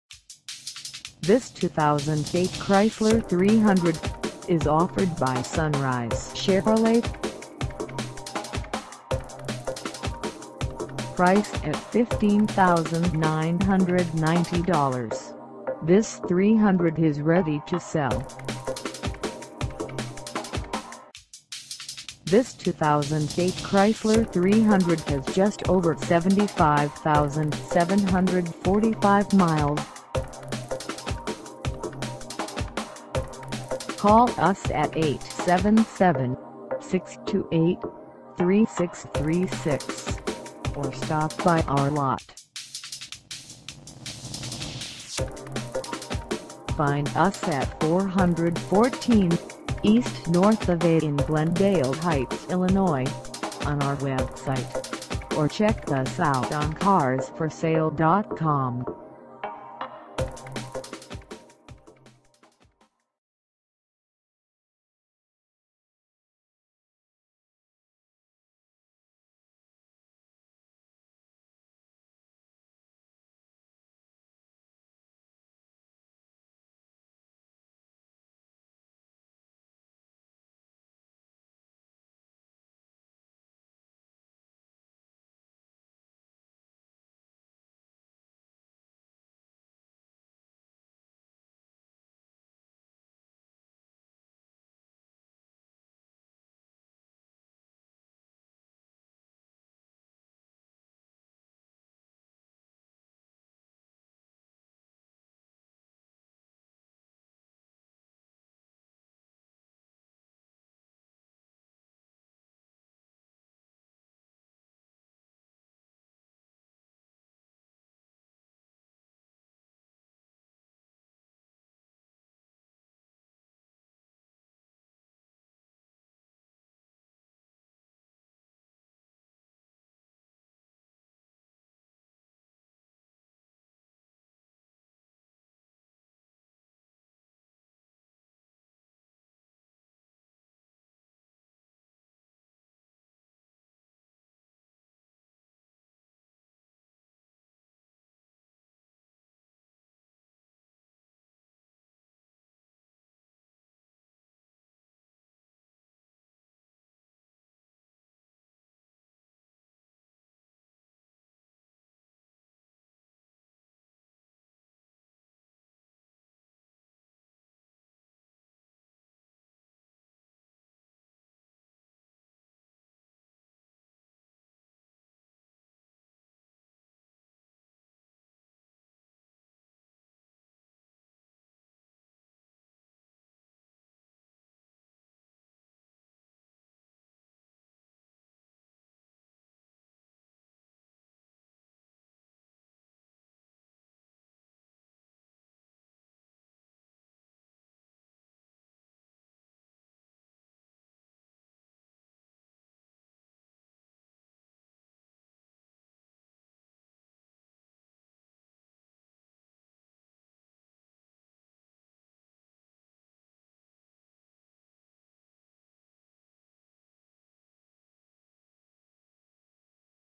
2008 Chrysler 300 Limited - for sale in GLENDALE HEIGHTS, IL 60139
SUNRISE CHEVROLET
414 east NORTH AVE

Come test dirve this 2008 Chrysler 300 Limited for sale in GLENDALE HEIGHTS, IL.

*** 2008 Chrysler 300 Limited *** ONE OWNER!! Popular Color and Option Package!! Nicely Loaded w/Auto Air Power Leather Seats Power Windows and Locks Keyless Entry Traction Control Premium Stereo w/SiriusXM Satellite Radio Capabilities and Much More!! Fully Inspected and Serviced By Our Factory Certified Service Center Professionally Detailed and Ready To Go!! Great Financing and Warranty options available on this one!! Carfax Report Purchased 09/10/2012 NADA Central 09/2012